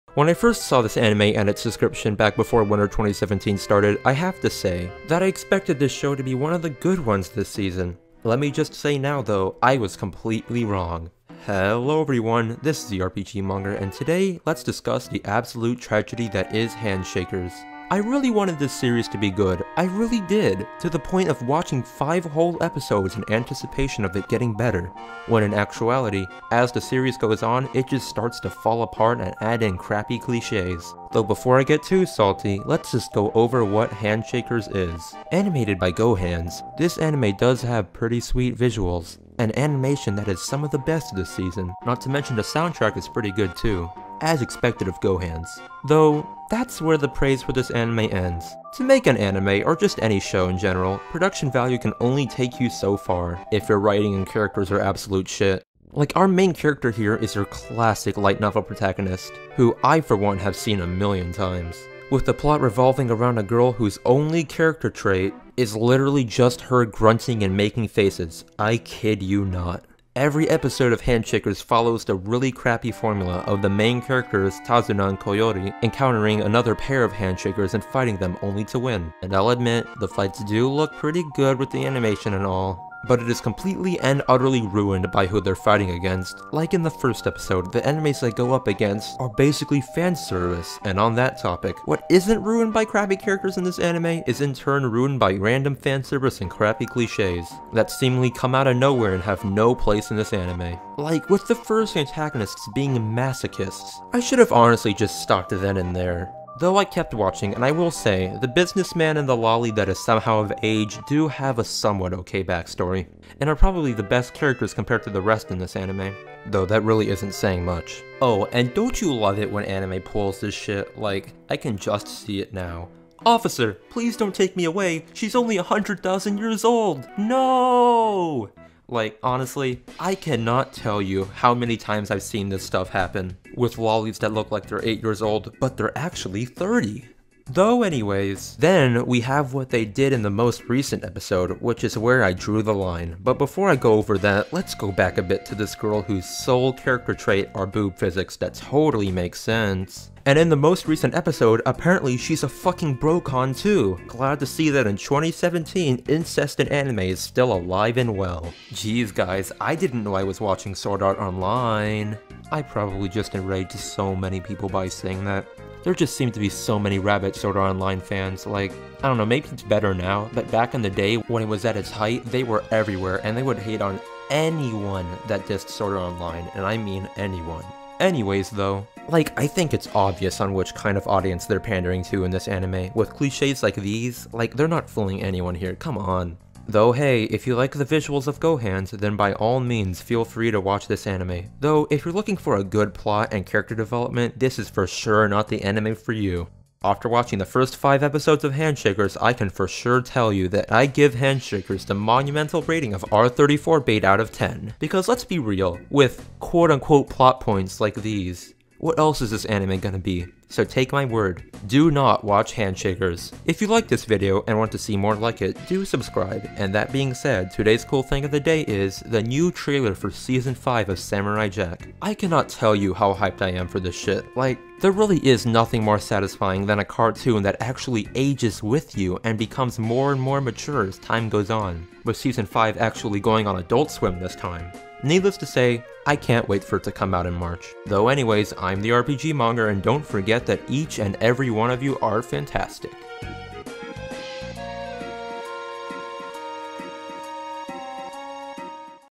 [OLD] Why You Shouldn't Watch Hand Shakers | First Reaction Episodes 1-5
Hand Shakers, probably the worst disappointment this season with little to no good aspects to it, though in what ways is it horrible?

Cool thing of the day: Samurai Jack Season 5